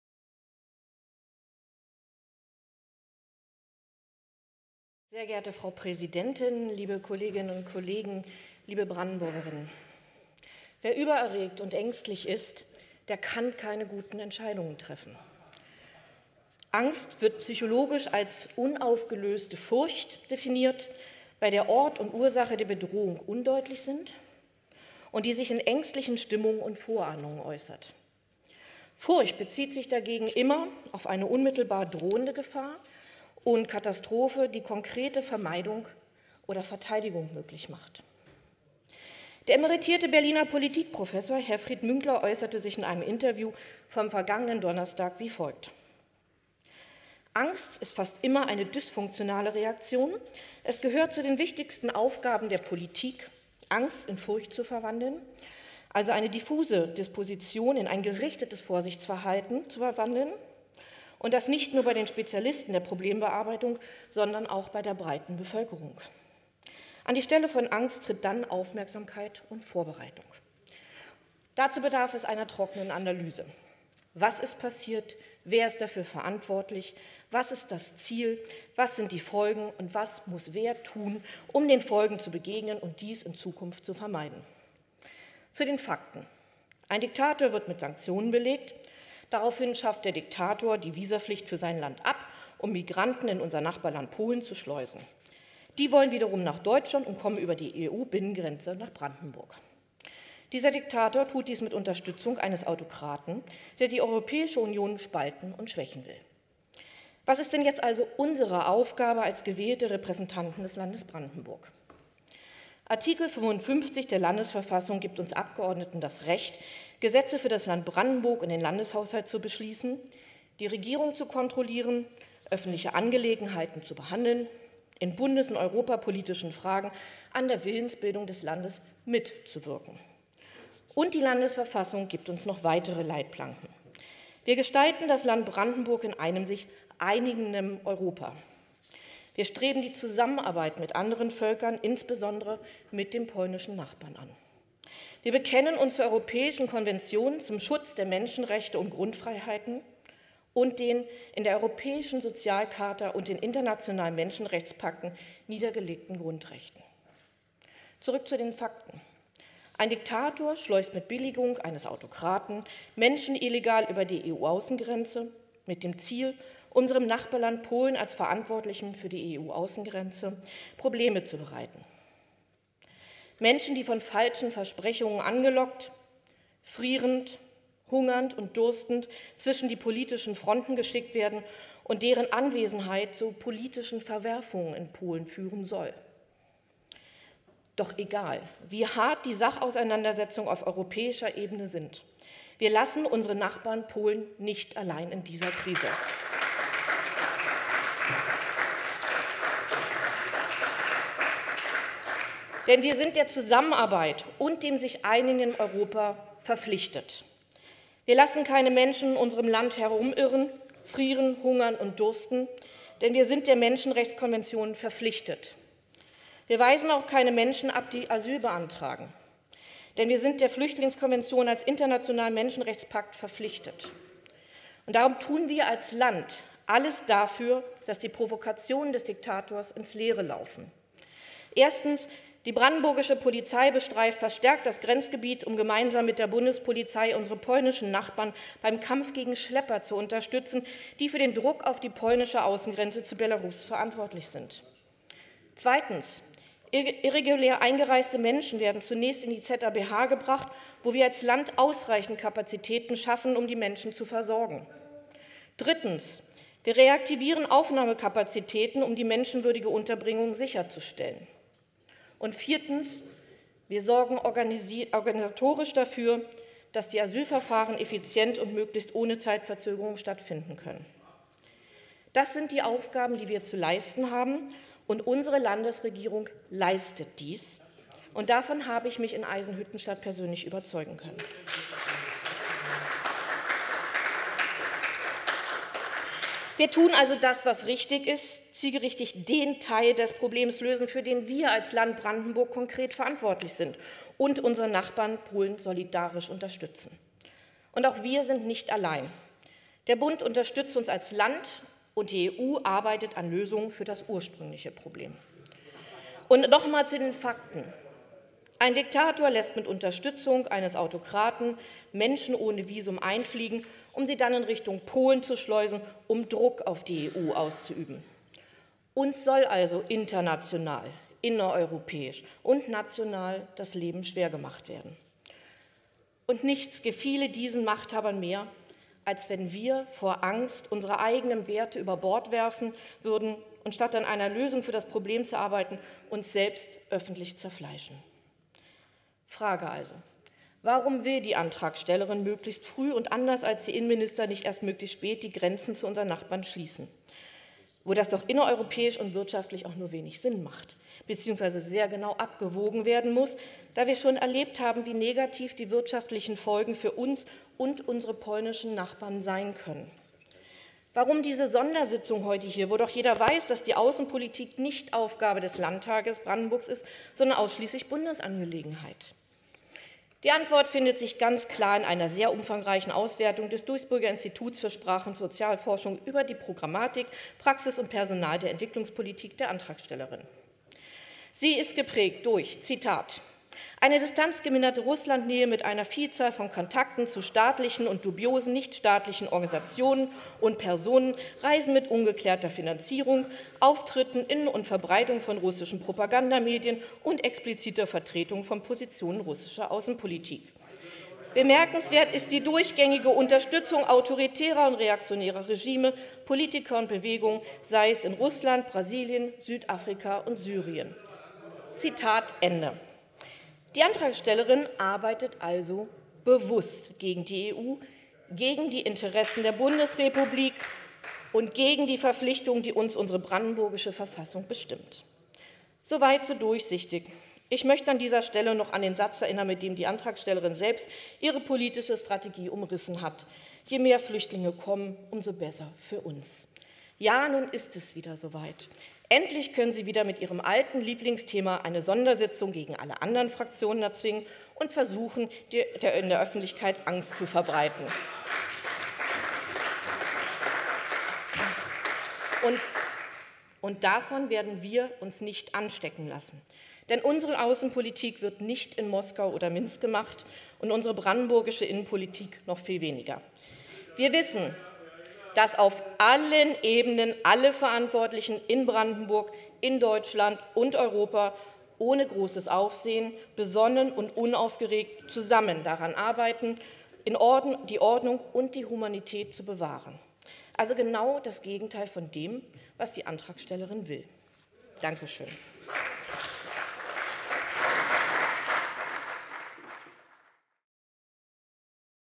Inka Gossmann Reetz: Wir dürfen unsere eigenen Werte nicht über Bord werfen!
Der Landtag ist heute zu einer Sondersitzung zusammengekommen, um über die aktuelle Migrationslage im brandenburgischen Abschnitt der deutsch-polnischen Grenze zu debattieren.
Der Diktator aus Belarus lässt Menschen einreisen um sie dann in Richtung Polen zu schleusen und damit Druck auf die EU auszuüben. Am bedauerlichsten daran ist, dass es wieder Menschen auf der Flucht sind, die zwischen die politischen Fronten geraten.
Wir dürfen nicht zulassen, dass Migrantinnen und Migranten zum außenpolitischen Spielball werden. Ebenso dürfen wir unser Nachbarland Polen in dieser Krise nicht alleine lassen. Wir sind der Zusammenarbeit und dem sich einigenden Europa verpflichtet.
Unsere Sprecherin für Innenpolitik Inka Gossmann-Reetz erklärt, welche Aufgaben das Land Brandenburg in diesen Zeiten zu leisten hat und welche Maßnahmen bereits unternommen werden.